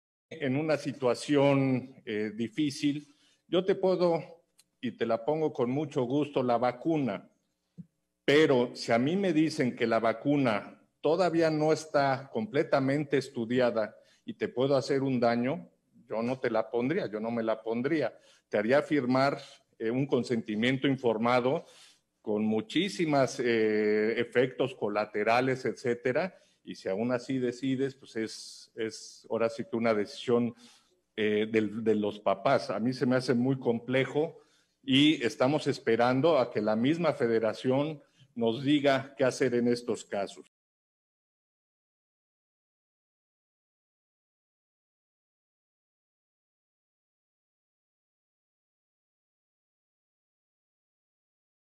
Cuatro padres han obtenido amparos para vacunar contra la covid a sus hijos menores de edad
El secretario de Salud, José Martínez informó que de proceder con la vacunación de menores, los padres deberán firmar una carta de consentimiento.